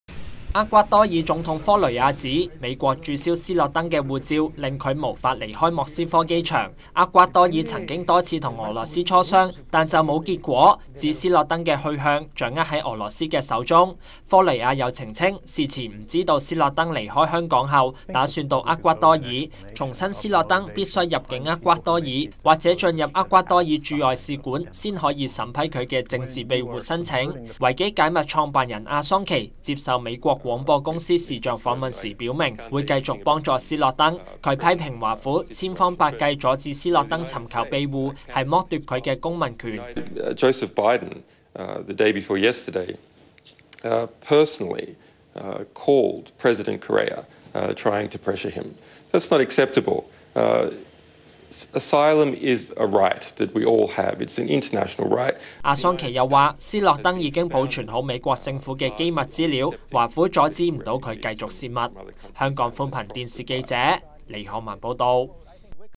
厄國稱俄掌控斯諾登前途
美國中情局前僱員斯諾登仍然滯留莫斯科機場過境區，有關他的政治庇護申請，厄瓜多爾表示全要視乎俄羅斯會否批准斯諾登進入厄瓜多爾大使館。